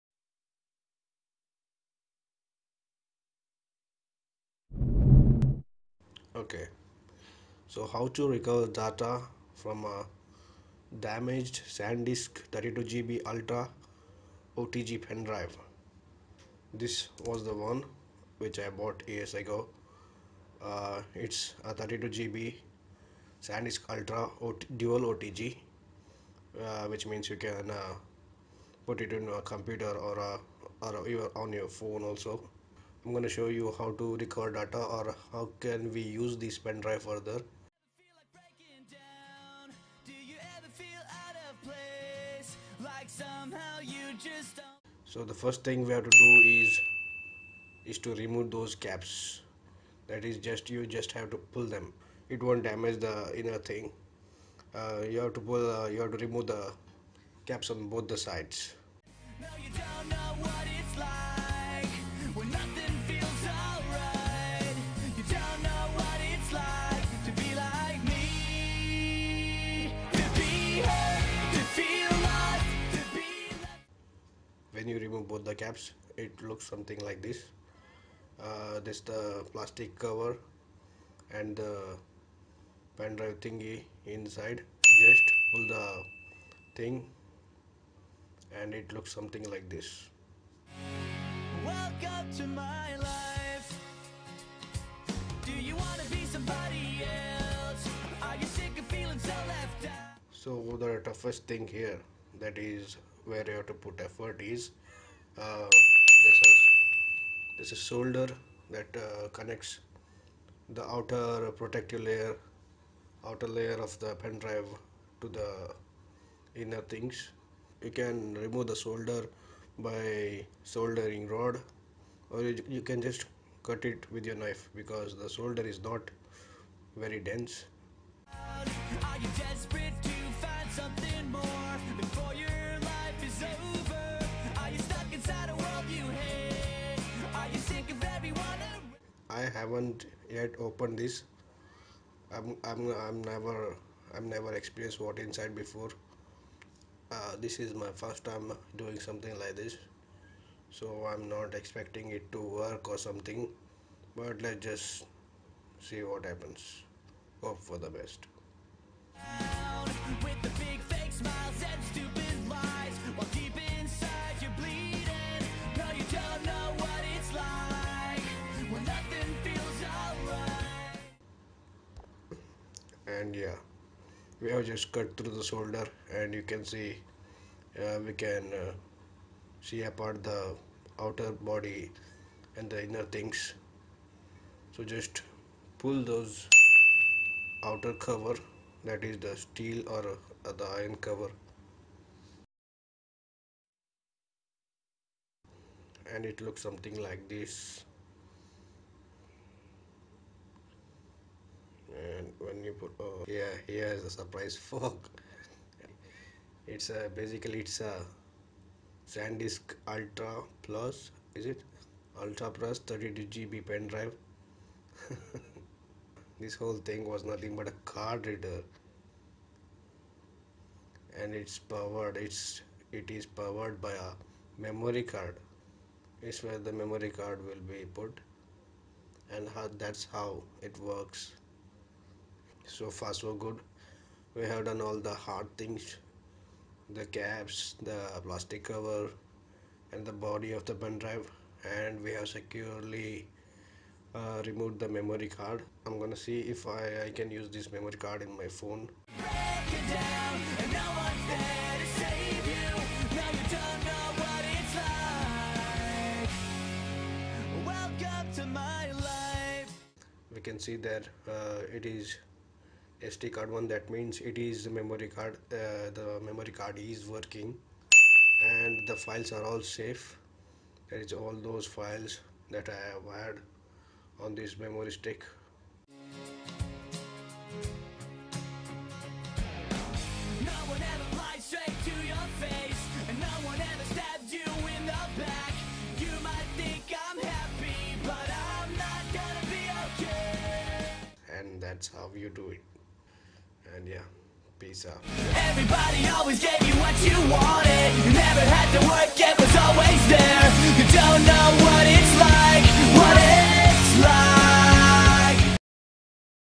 How to Repair Corrupted Pendrive | Sandisk Dual OTG | USB Not Working | Data Recovery
Revelation | SanDisk Dual USB Drive 3.0 | What's Inside | Recover Data

SanDisk Ultra  Dual USB Drive 3.0 for Mobile & PC Speed up-to 150MB/s
Kick-ass pen drive at Size, Speed and Style but didn't last even 12 months. Couldn't dispose it because i had some of my important data in it. I find something awesome inside when i dissimulate it.
Though the drive cannot be rebuilt, the files can be recovered and the damaged pen drive can be used as a memory card

P.S I felt bit conned because technically this is a Card Reader